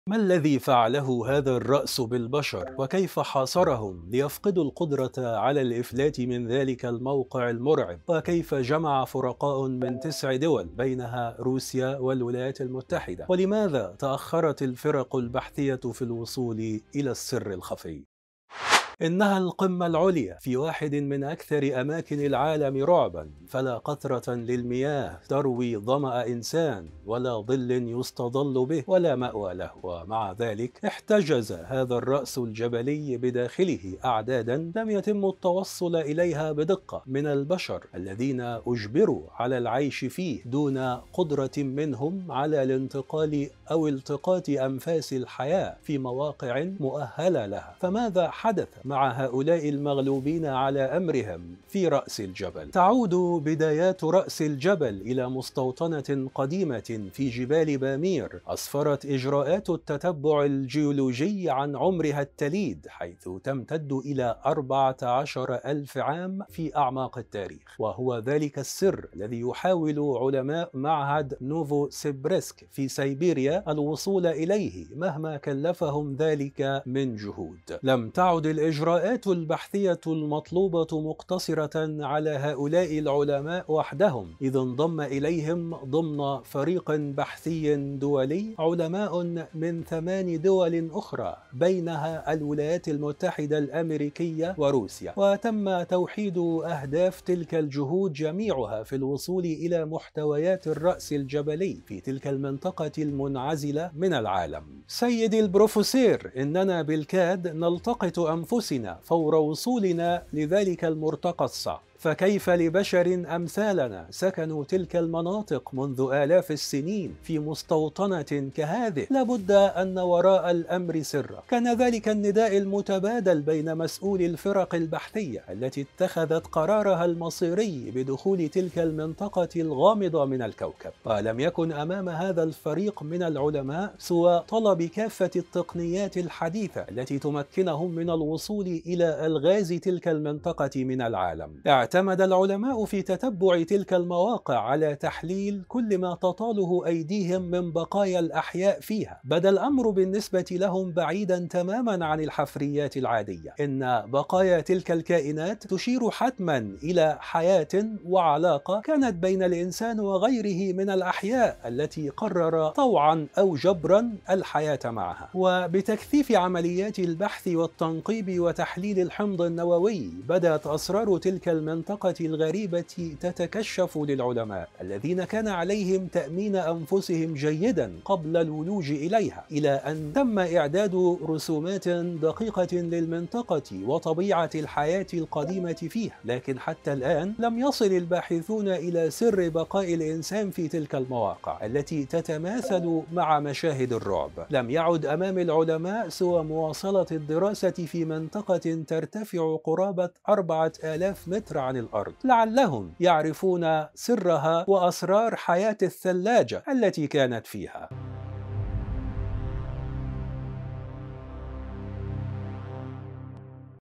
رأس جبل الأبدية.. عمرها 14 ألف عام .. ما هي المخلوقات التي عاشت مع البشر والأسرار الغامضة
ما الذي فعله هذا الرأس بالبشر؟ كيف حاصرهم ليفقدوا القدرة  على الإفلات من ذلك الموقع المرعب؟ وكيف جمعت فرقاء من تسع دول بينها روسيا والولايات المتحدة؟ ولماذا تأخرت الفرق البحثية عن الوصول إلى السر الخفي؟